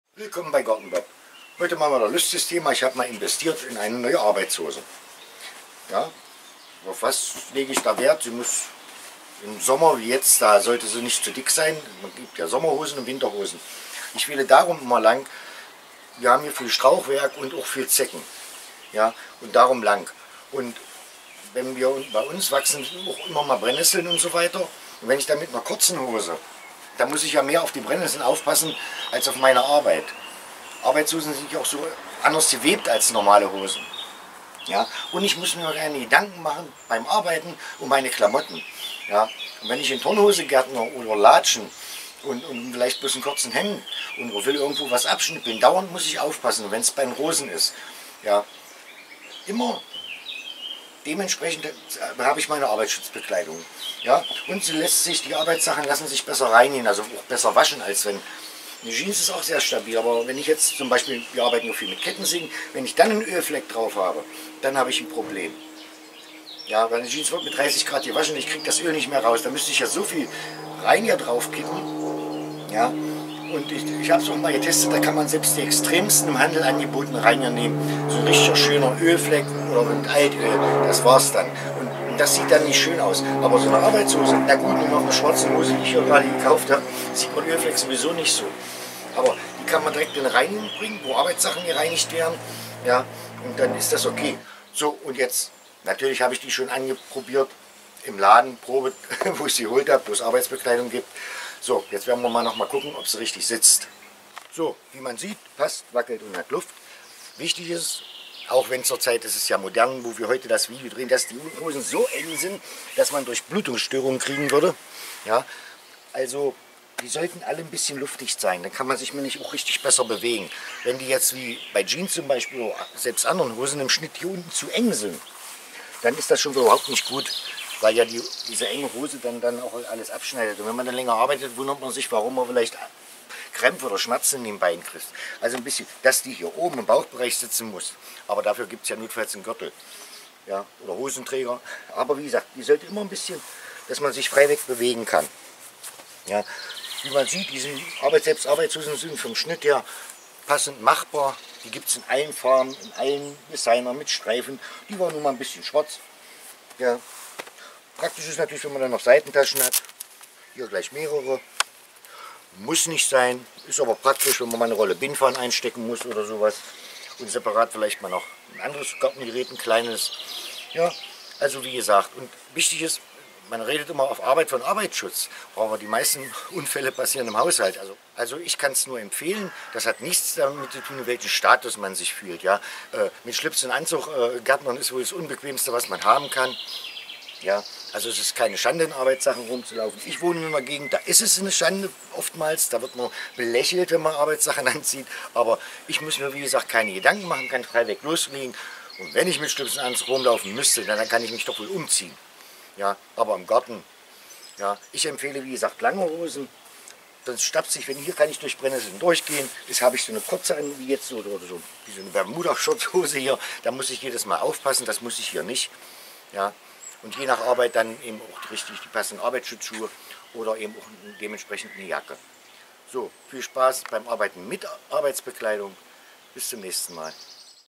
Arbeitshose für den Garten
Es würde beim mir Zeit für eine neue Arbeitshose für den Garten. Lohnt sich überhaupt eine extra Arbeitshose? Auf jeden Fall, denn schließlich lässt sich damit im Garten, der Werkstatt oder am Haus deutlich besser arbeiten als mit einer einfachen Jeans oder kurzen Hose.

Bei der Auswahl der richtigen Hose achte ich besonders auf den Tragekomfort sowie deren Verarbeitung, denn schließlich soll diese eine Weile halten.